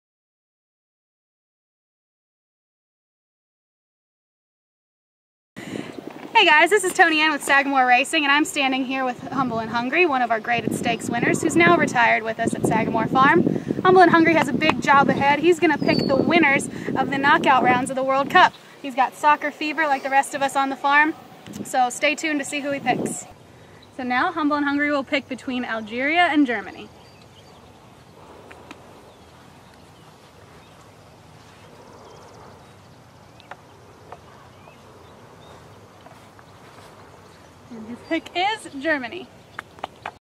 Humble and Hungry Predicts the 2014 World Cup: Algeria vs Germany
Newly retired Sagamore Racing thoroughbred Humble and Hungry has taken up a new interest - World Cup predictions! See his pick for the Algeria vs Germany match on Monday, June 30!